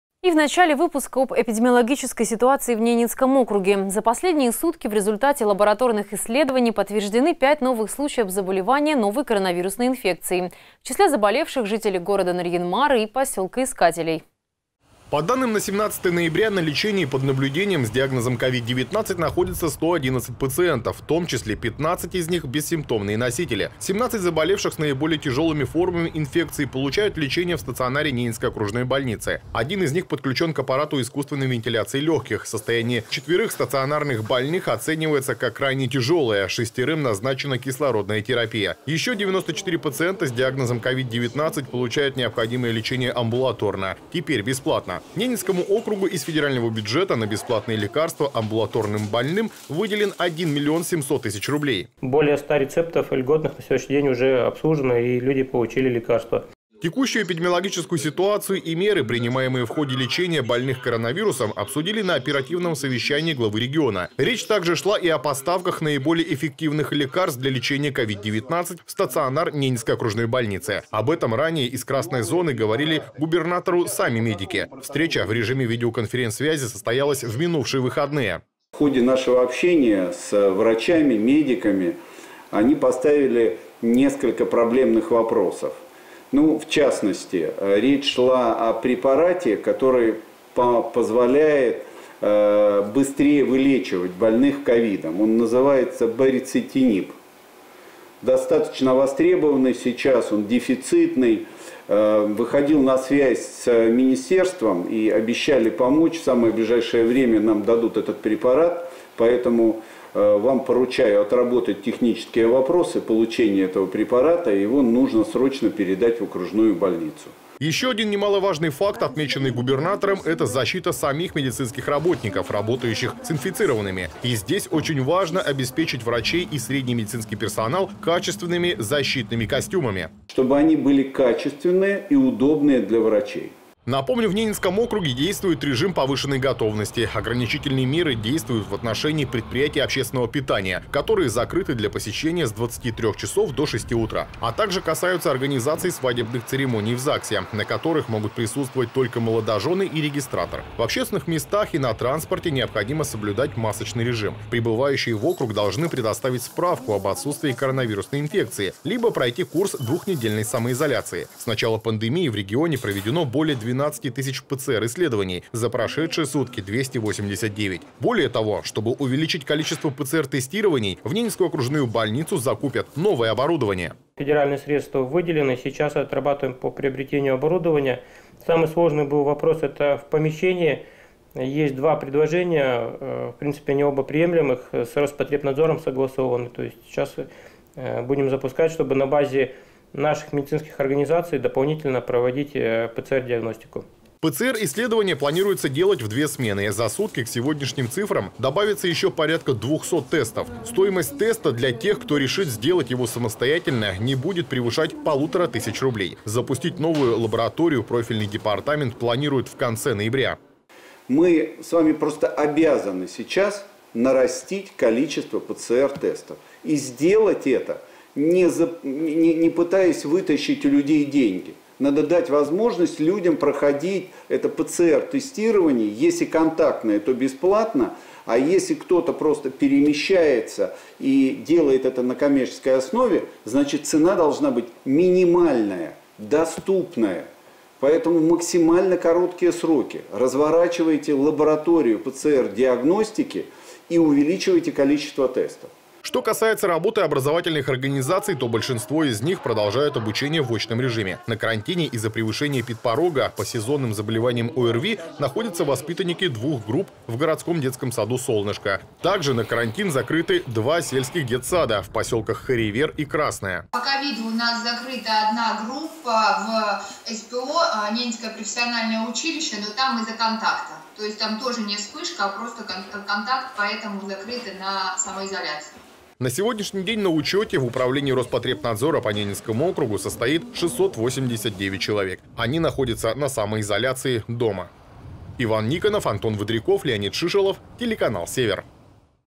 ЭПИДОБСТАНОВКА В НЕНЕЦКОМ АО
Эпидемиологическая ситуация в Ненецком автономном округе. За последние сутки в результате лабораторных исследований подтверждены пять новых случаев заболевания новой коронавирусной инфекцией. В числе заболевших - жители города Нарьян-Мара и поселка Искателей.